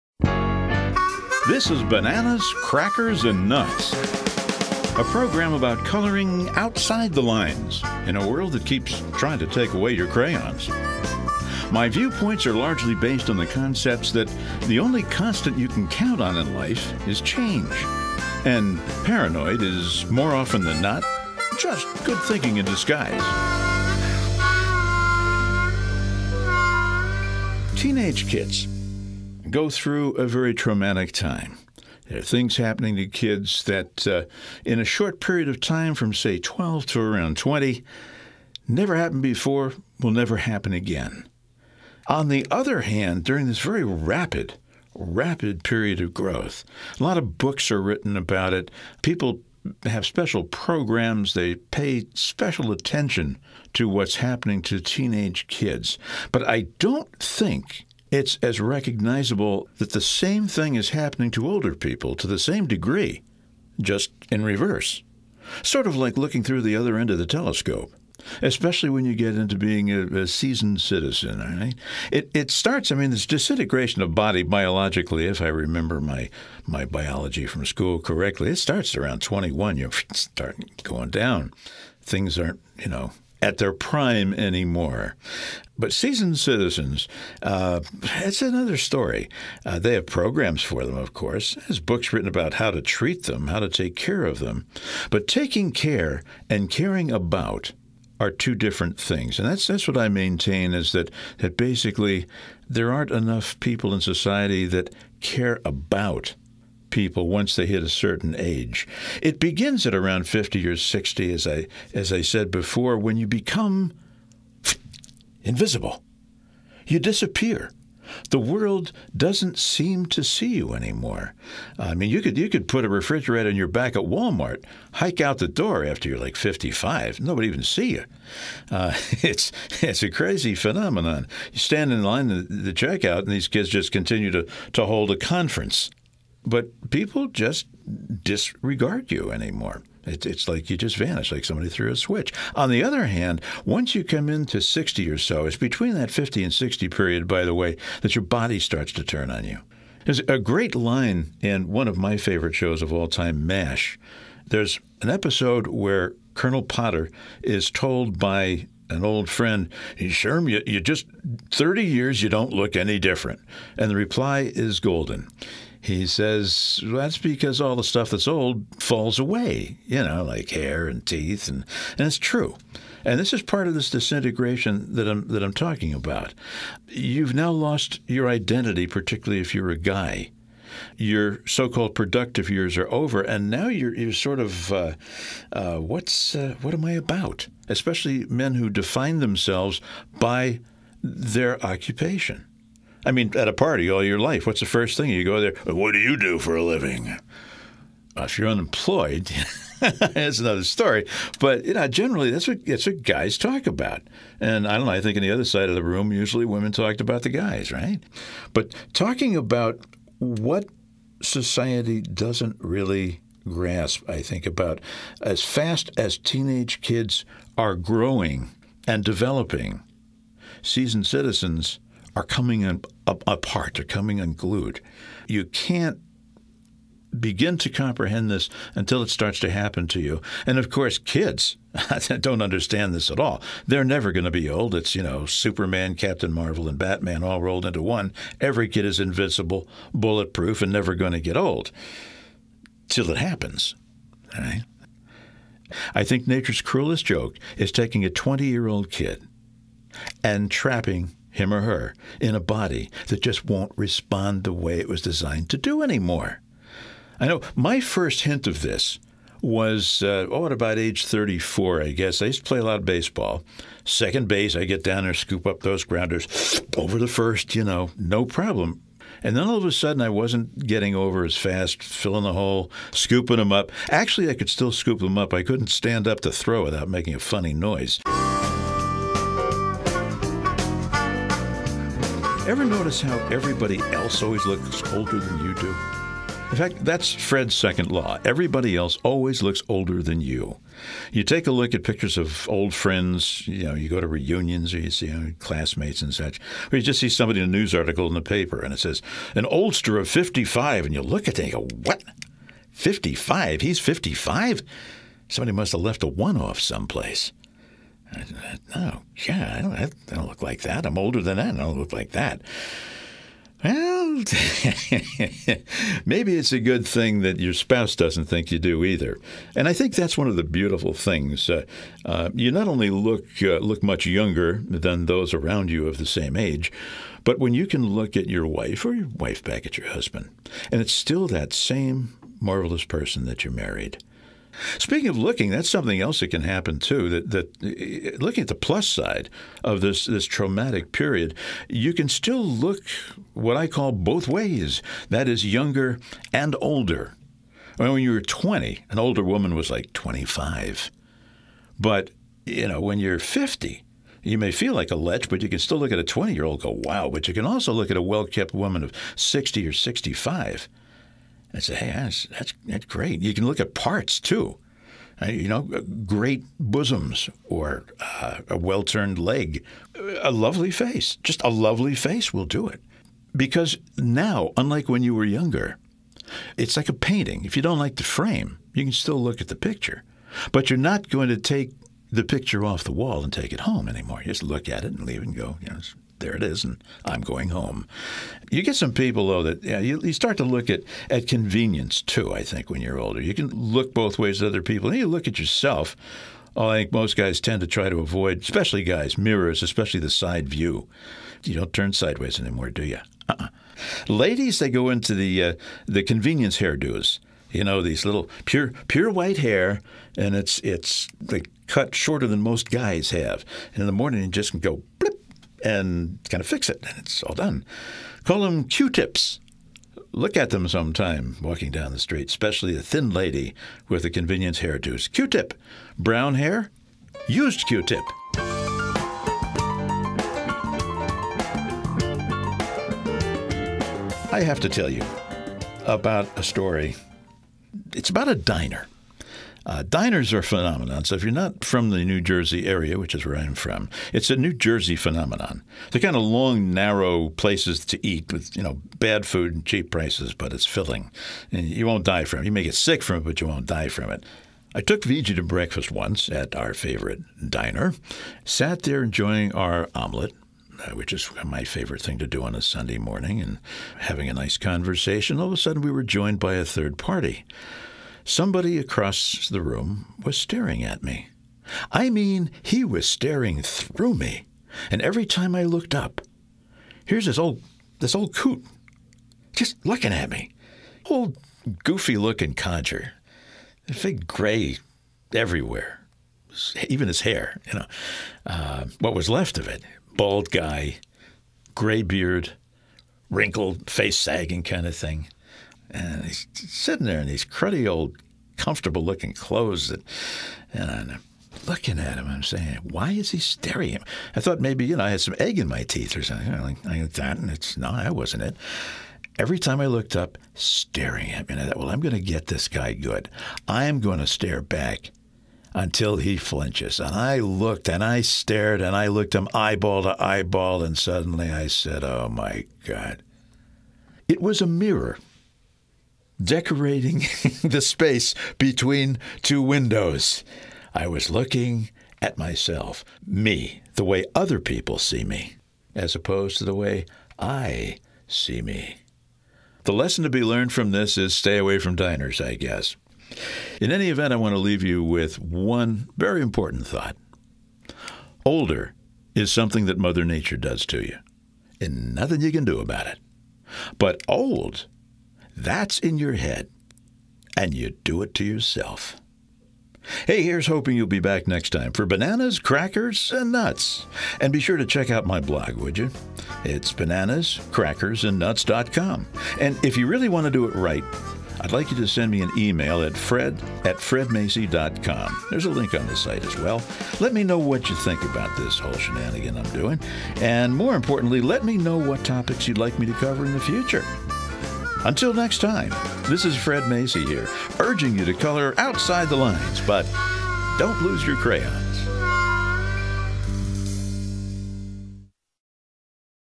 Other End of the Telescope
The good, the bad and the ugly of adjusting to the changes that occur during our Senior years vs. the trauma of being a Teen.